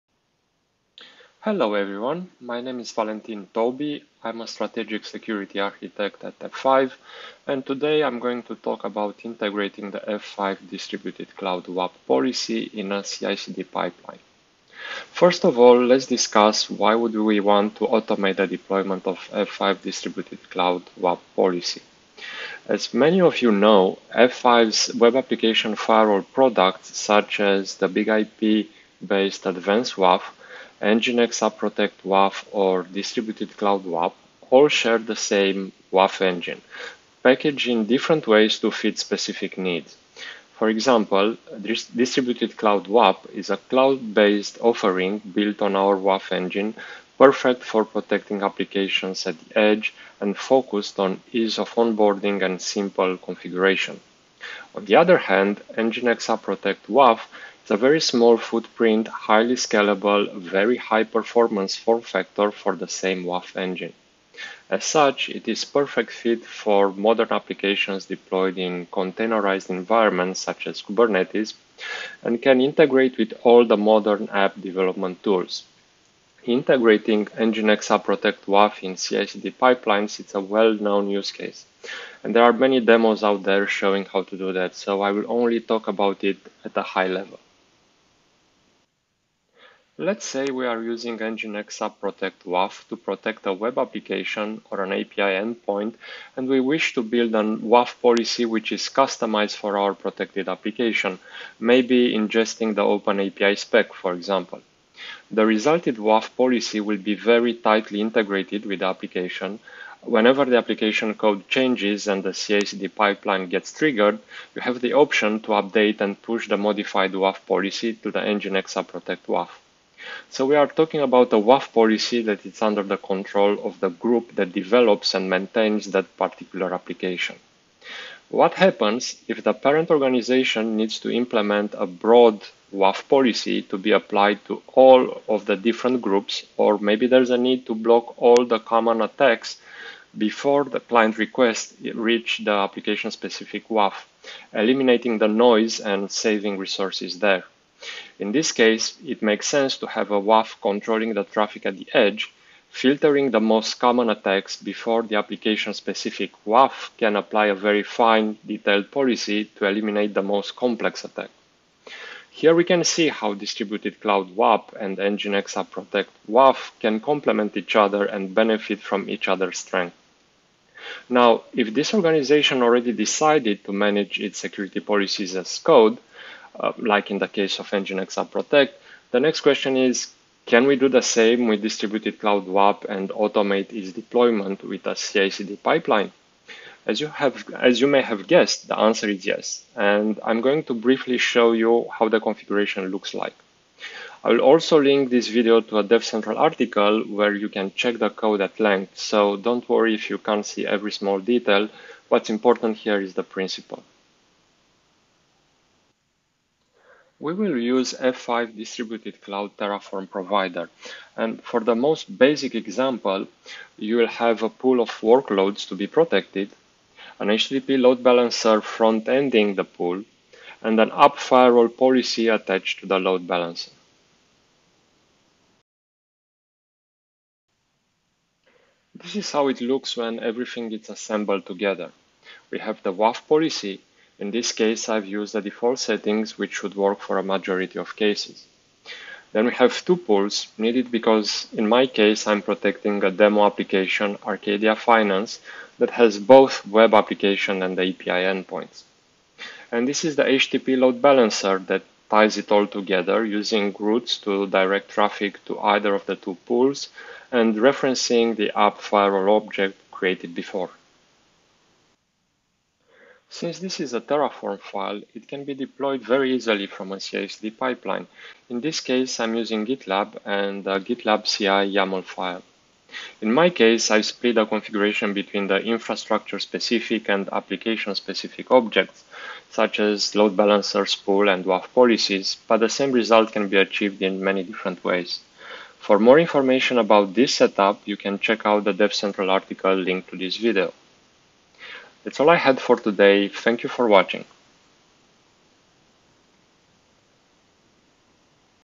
F5 Distributed Cloud WAAP Hybrid Architecture for DevSecOps
In this video, Valentin Toby, a strategic security architect at F5, discusses the integration of F5 Distributed Cloud Web Application and API Protection (WAAP) into CI/CD pipelines. He explores the importance of automating security policies within DevSecOps practices, highlighting the benefits of a hybrid architecture for enhanced security and efficiency. Viewers will gain insights into the role of automation in modern development workflows.